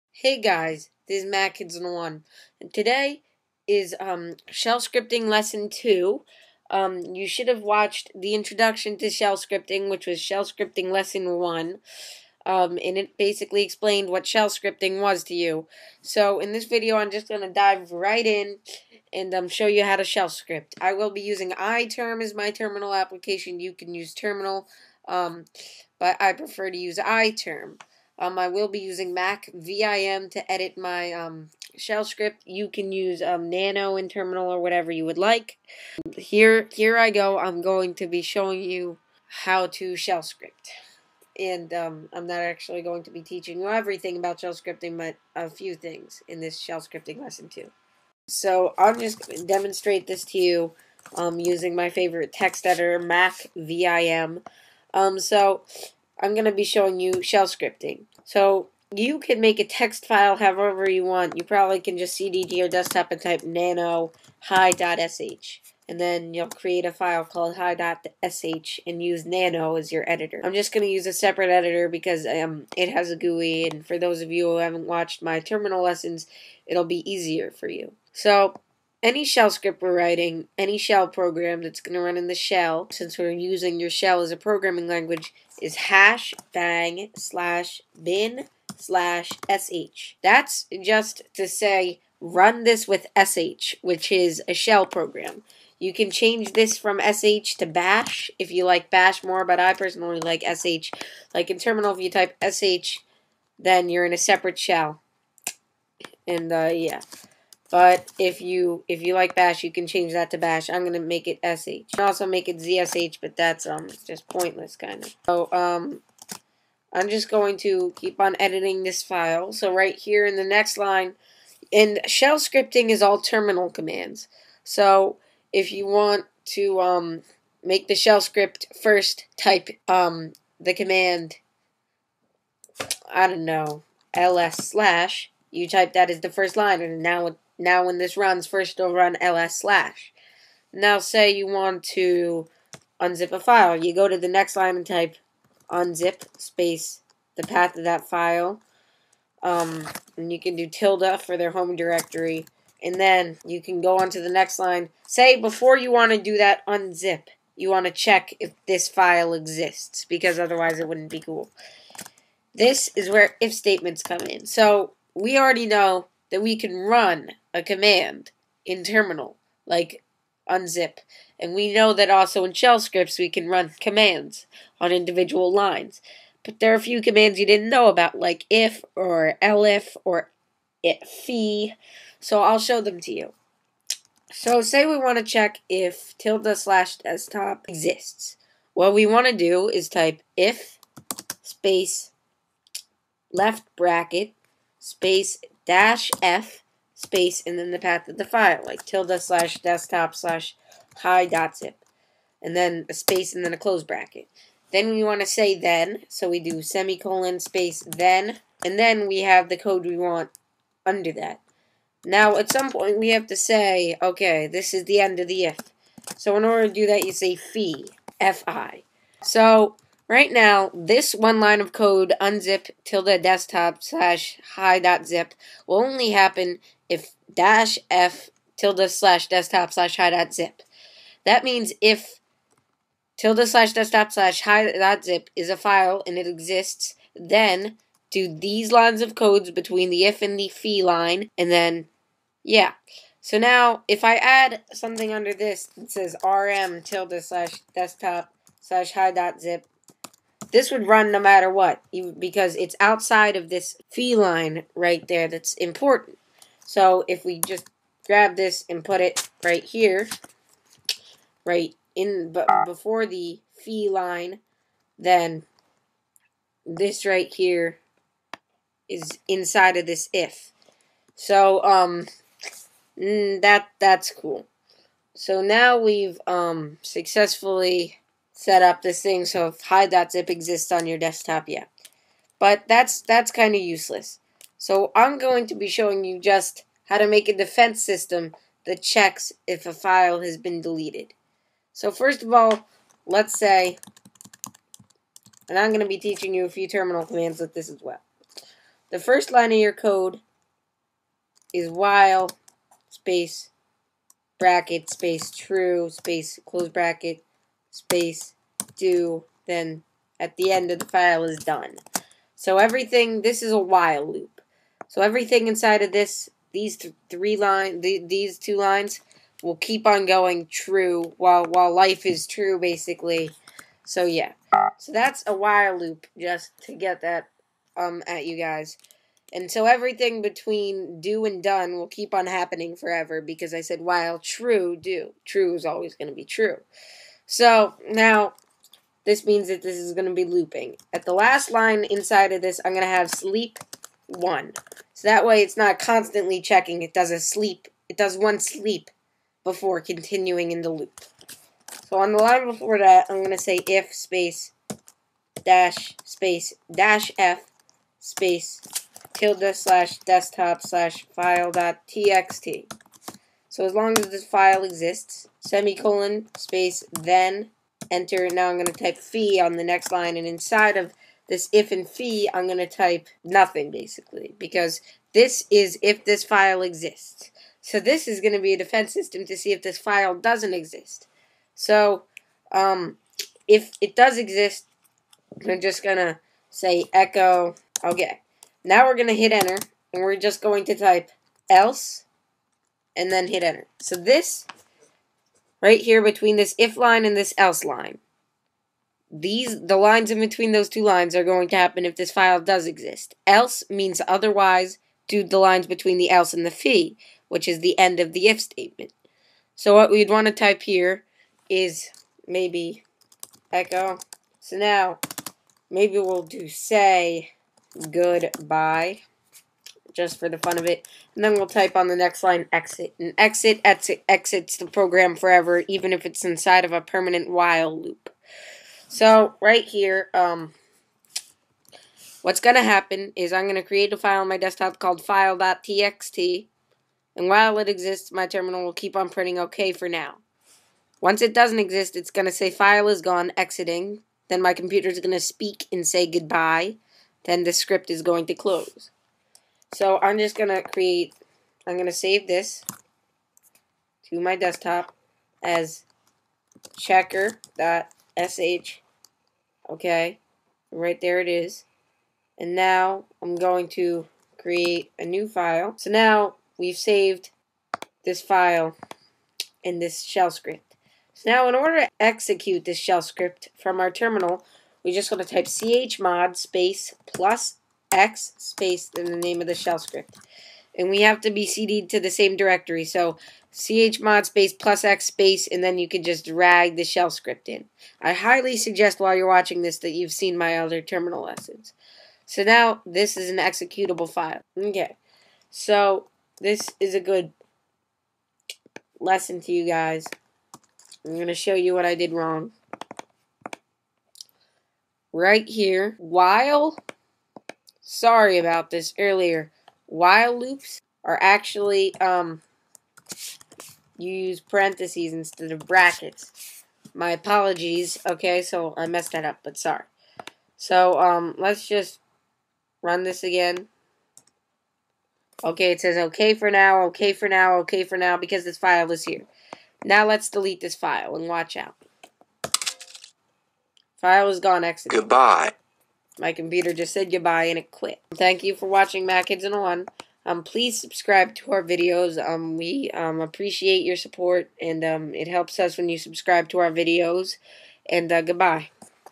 Shell Scripting Lesson 2
I show you how to make a simple shell script